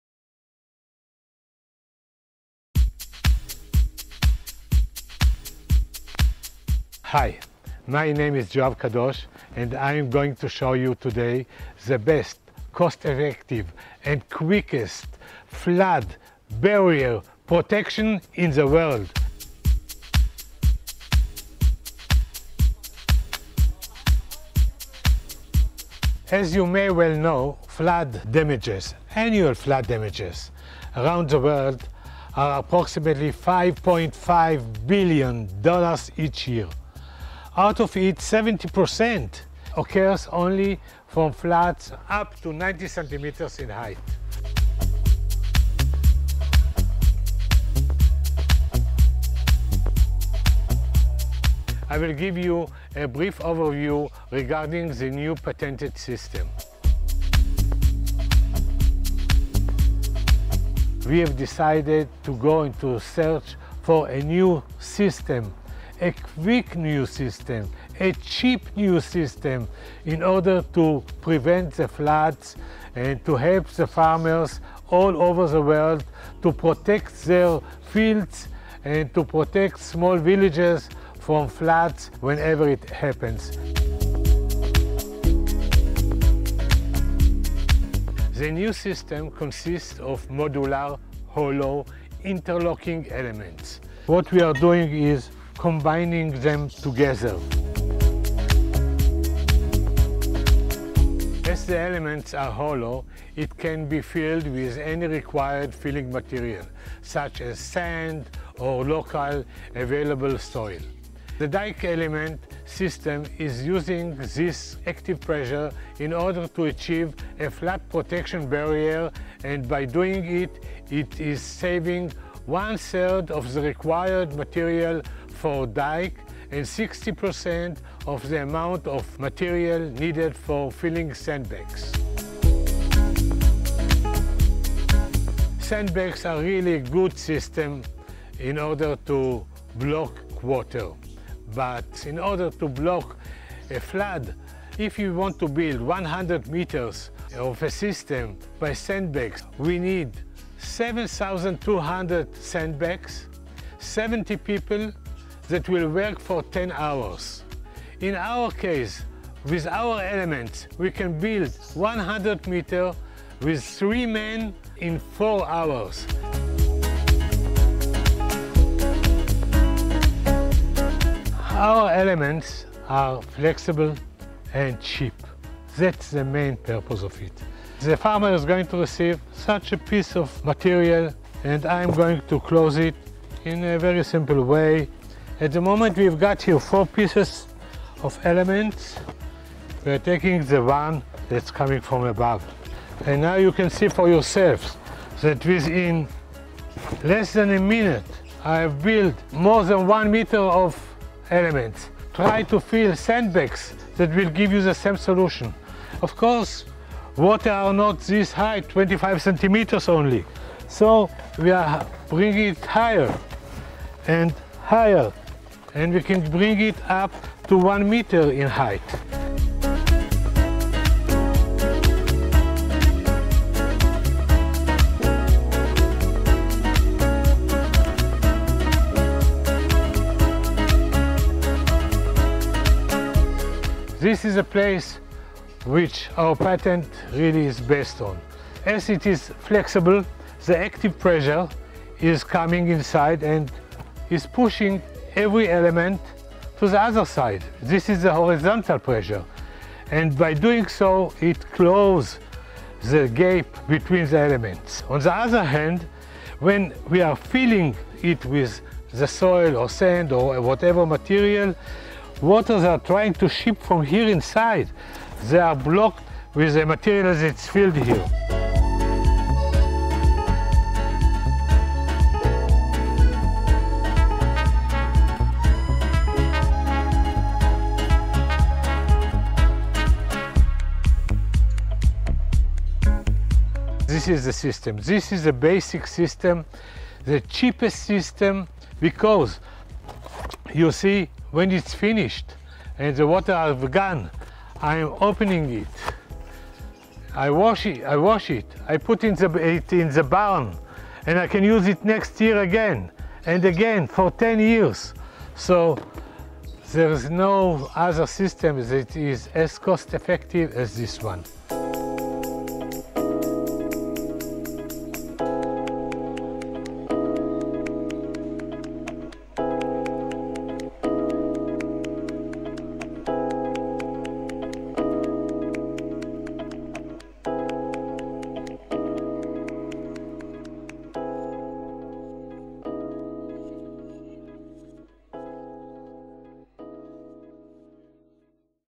flood walls protection
The quickest, the cheapest and the best cost effective patented flood barrier for agriculture land, and villages.
Best protection for your fields and houses.